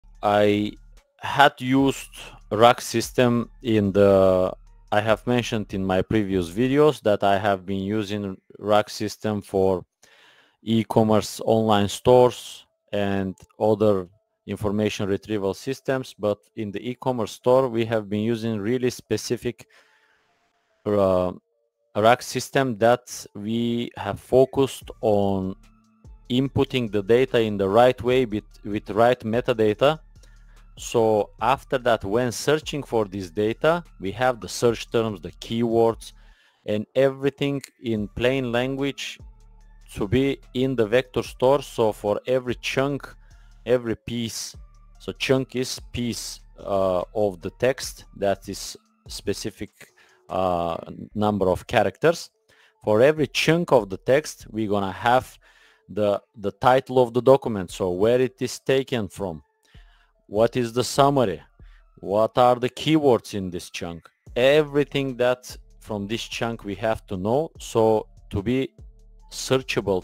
How to structure RAG data for VectorSearch – chunking + metadata for ecom
Learn to automate anything with free tools. Tutorials, templates, and a community of builders.